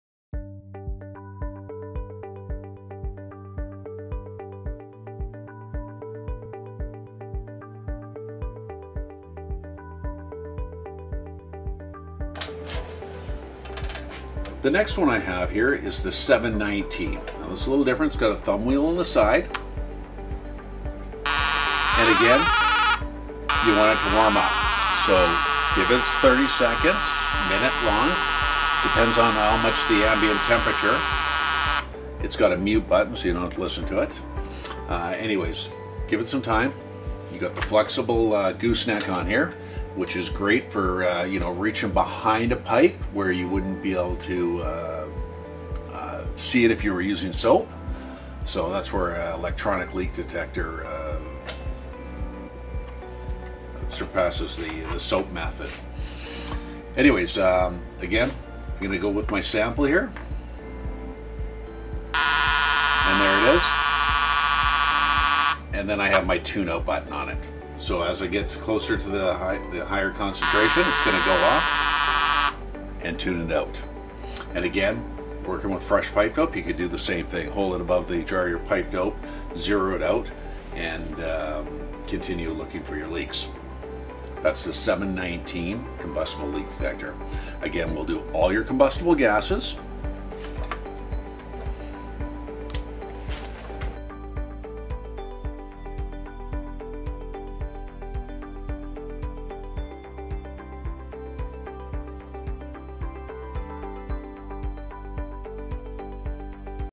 TPI 719 Combustible Gas Leak Detector
The 719 offers 30ppm sensitivity to detect combustible gasses.

Easily operated with one hand, leaks are pinpointed using the audio and visual indicators. An adjustable “tic” rate helps eliminate background gas concentration in contaminated environments.

Instant response
30 ppm (methane) sensitivity
Adjustable tic rate
One-hand operation
Visual and audible leak indication
16” gooseneck
Soft carrying pouch